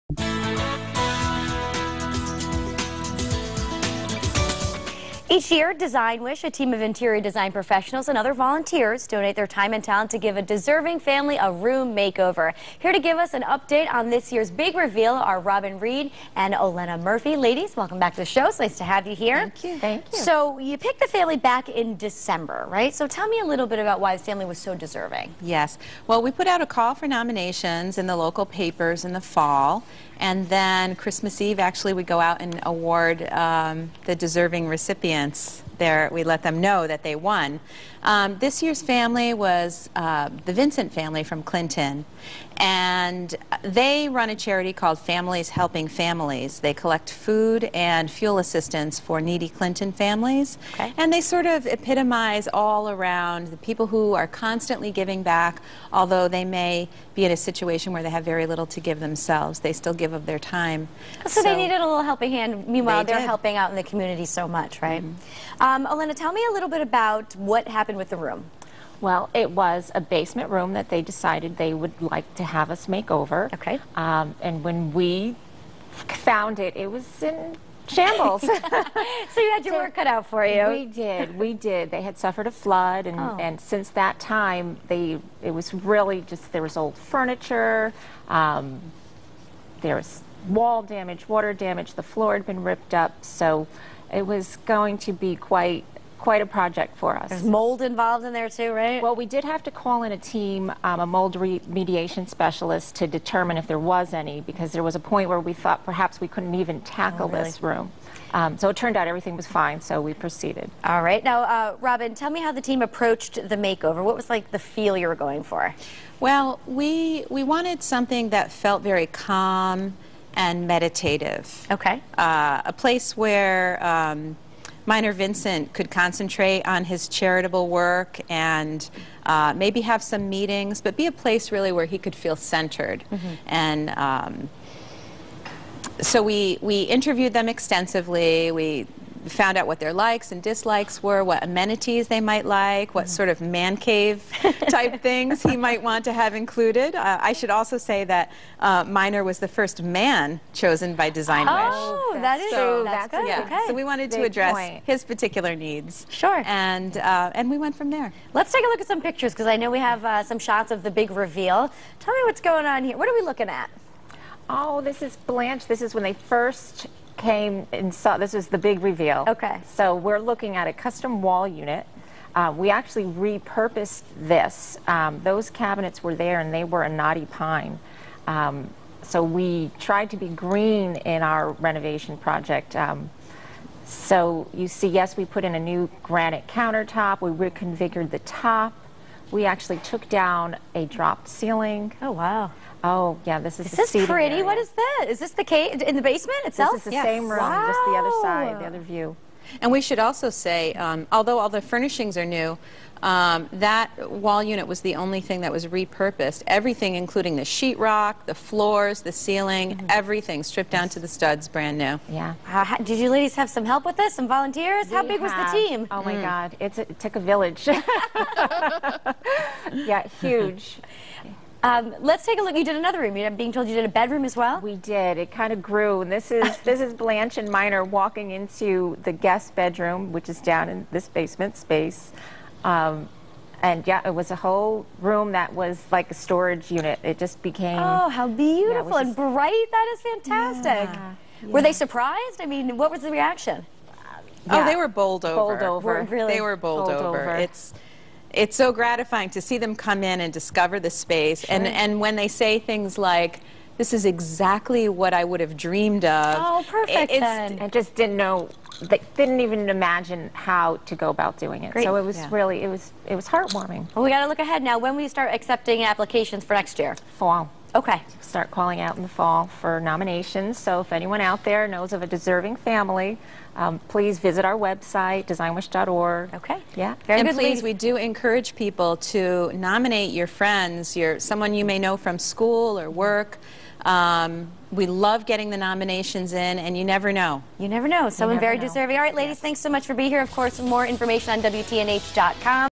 Design Wish grants a room makeover
DesignWish designs a room makeover for a deserving family.